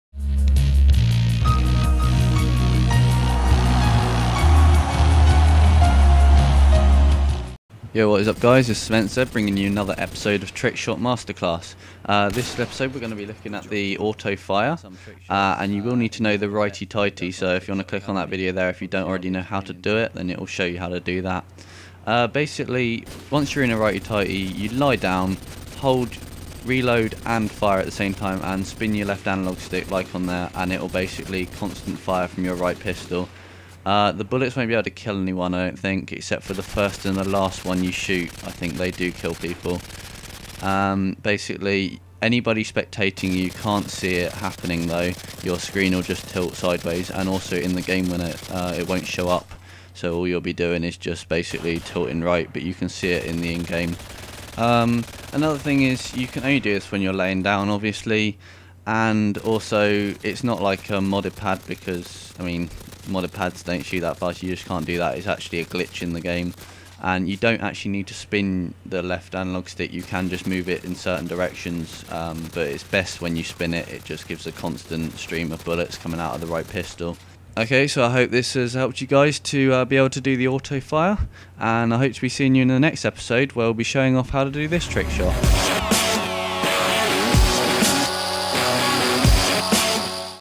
Trickshot MasterClass - Auto Fire
Immense Z.
Trickshot Masterclass.
Auto Fire Trickshot Tutorial.

Trickshot Masterclass Is an In-depth Trickshot Tutorial Series.
Furthering the explaination of shots that are in Trickshot Academy.
Hopefully the voice explainations in these tutorials and more time to explain each shot should help out more.
(All private match kills obviously).
No need to learn how to play claw, all trickshots will be done holding the controller normally.
Shots will range from beginner to advanced tricks.

Button Layout - Tactical
Sensivity - 9

Request - Please comment with any requests of shots you would like to see a tutorial for.